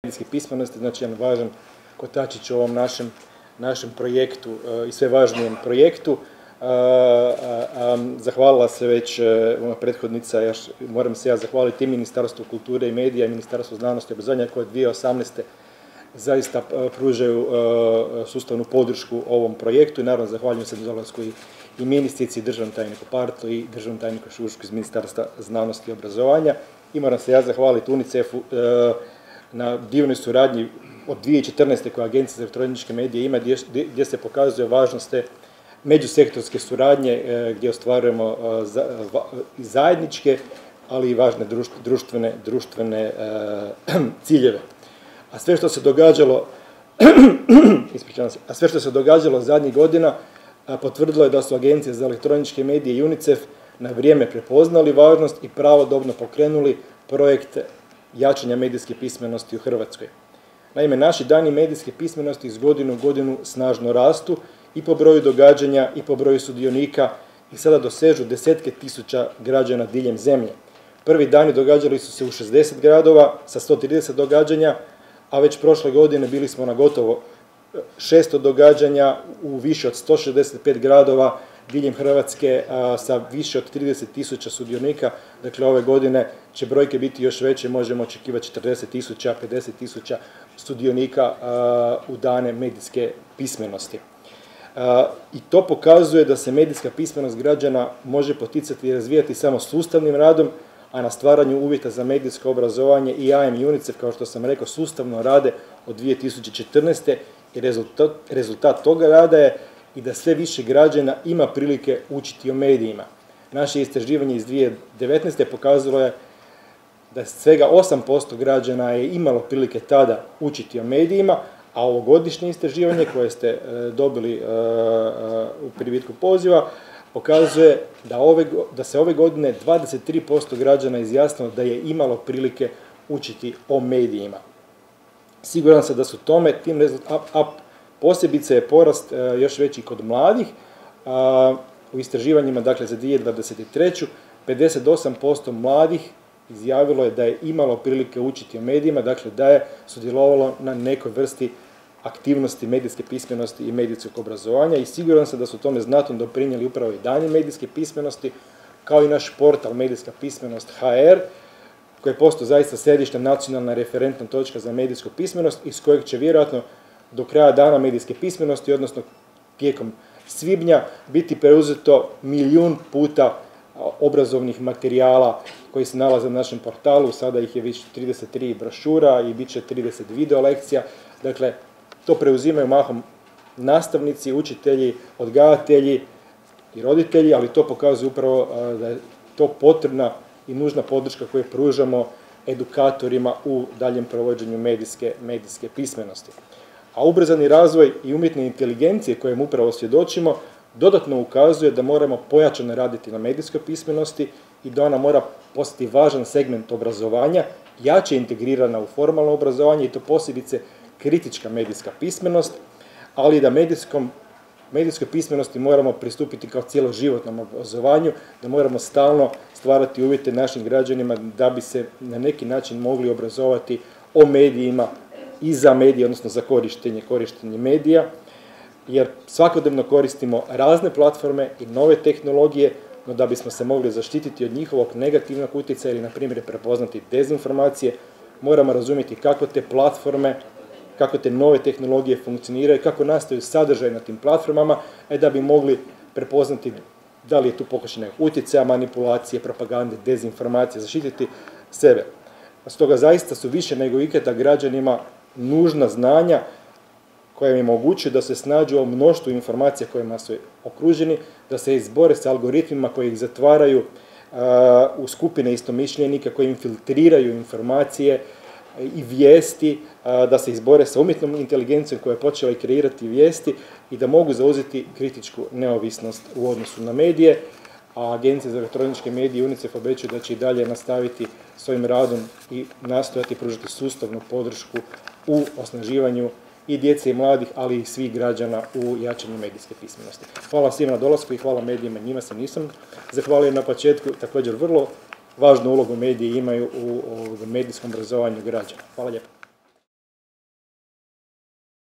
6. Dani medijske pismenosti - Tomljenović
Zagreb, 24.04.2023. - Konferencija za medije povodom početka 6. Dana medijske pismenosti koji će se održati od 24. do 30. travnja u organizaciji Agencije za elektroničke medije i UNICEF-a, a pod pokroviteljstvom Ministarstva kulture i medija te Ministarstva znanosti i obrazovanja
video HINA/ Edvard ŠUŠAK/ es